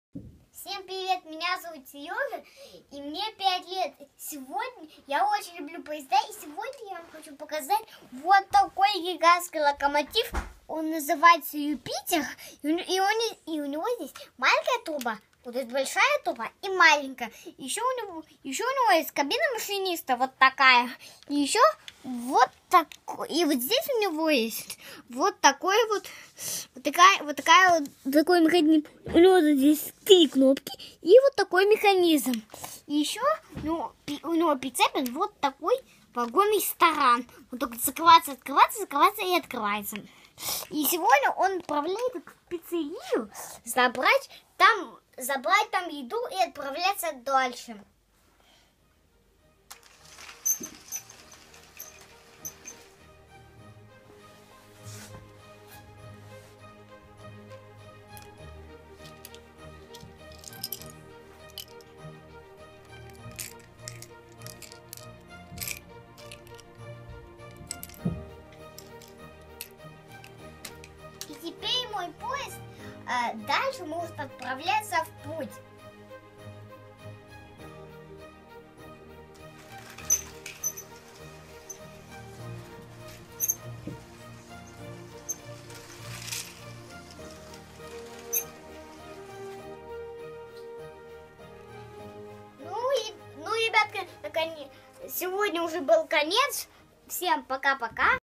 StartUp.Robo. Плешаков Сергей Сергеевич, Поезд Юпитер. Дошкольники.
Назначение: Современная модель поезда, ранее используемого для дальних путешествий по Америке в IX веке
Устройство: Состоит из двигателя от lego поезда, набора кубиков из других конструкторов и рисунков, изображающих внутреннее устройство поезда
Принцип действия: Поезд управляется через приложение Lego Powered Up. Состоит из двух программ: первая - подъезд к пиццерии за провиантом, вторая - отправление в дальнее путешествие.
Применение: Применяется для перевозки пассажиров и грузов
Уникальность: Собственная разработка современной модели поезда с программным управлением
Ссылка на голосование:
Хочешь так же создать свой проект и поучаствовать в конкурсе, переходи по ссылкам: